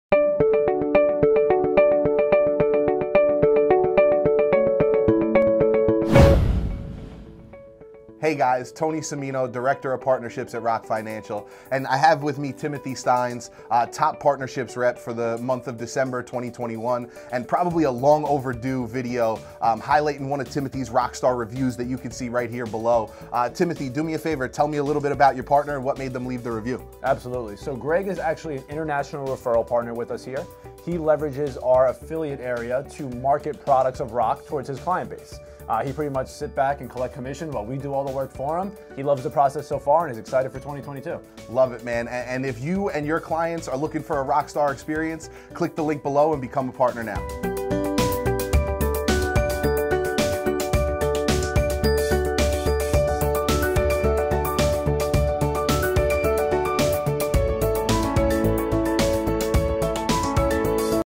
Getting Another 5-Star Review with Tony Cimino and Timothy Stines
There's nothing sweeter than knowing you're clients are happy!
Director of Partnerships Tony Cimino and Business Development Officer Timothy Stines highlight how one partner is seeing success and is ready to take on 2022!

Learn more about our Partnership Program and start earning a commission now